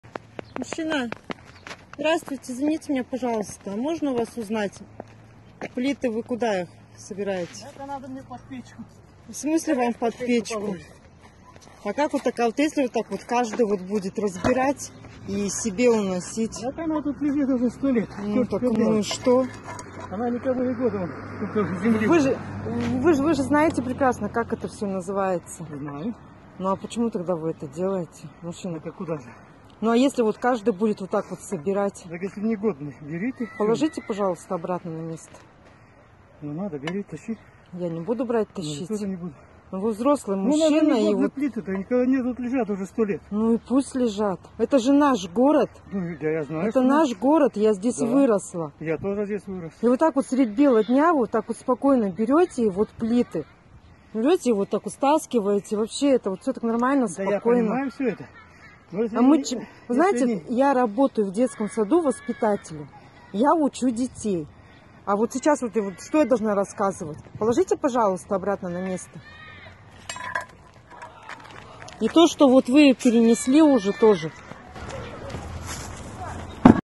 Мужчина воруем плиты в Краснокаменске
Видео взято с ТГ-канала "Инцидент. Краснокаменск"